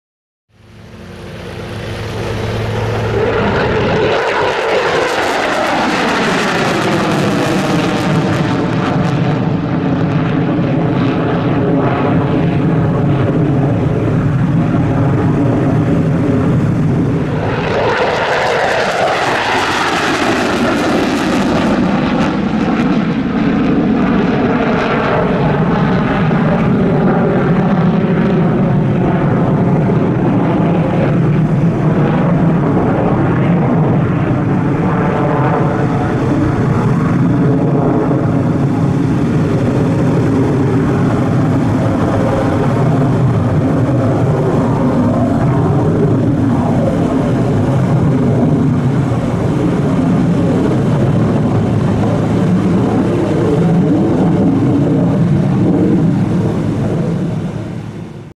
Quick Climbs of two F 15 Eagles
Quick climb departures of two F-15 Eagles of the 104th Fighter Wing in Westfield, Mass.